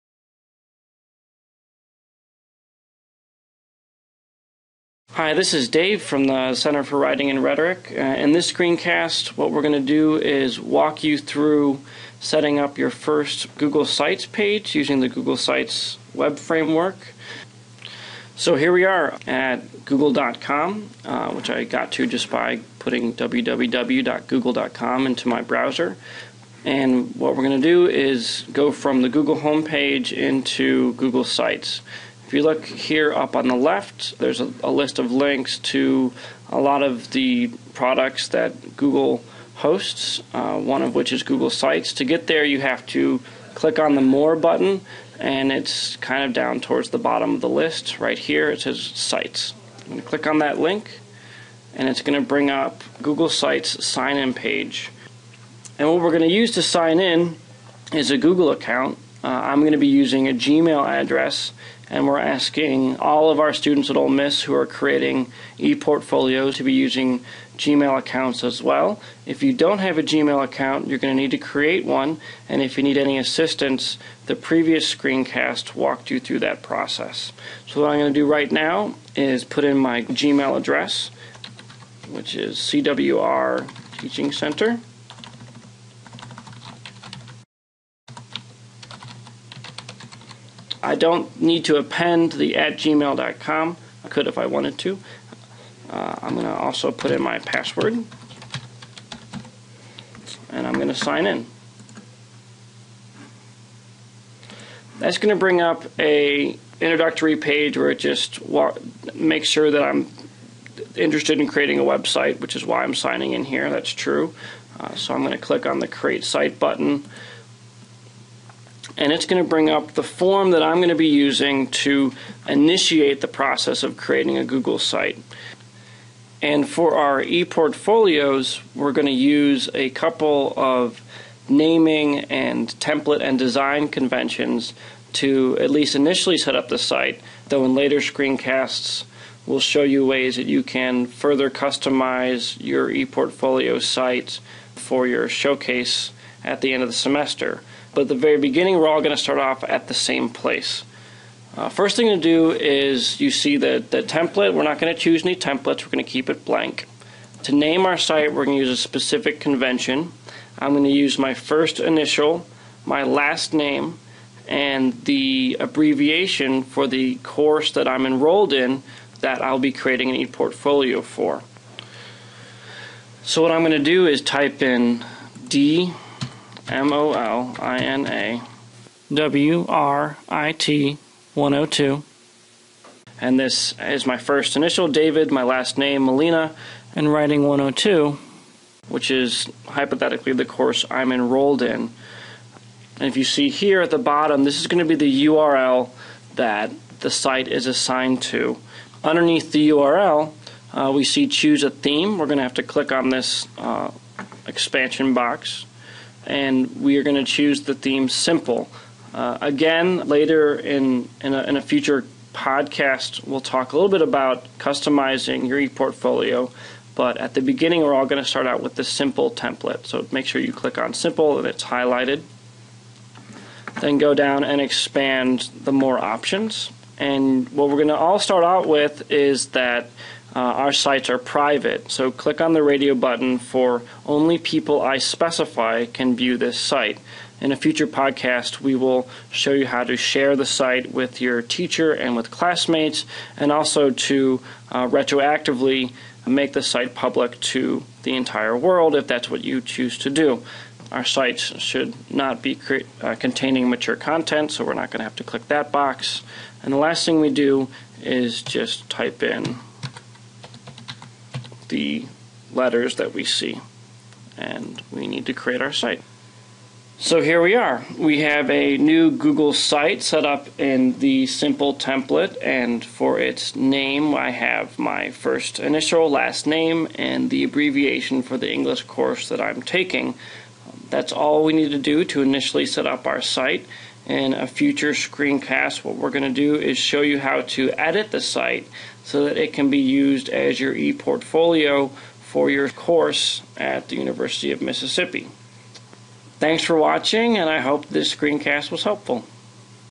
CWR: Getting Started with Google Sites
In our second introductory screencast, we walk you through the process of setting up a Google Site, which you will use to develop your ePortfolio for the Spring 2011 semester.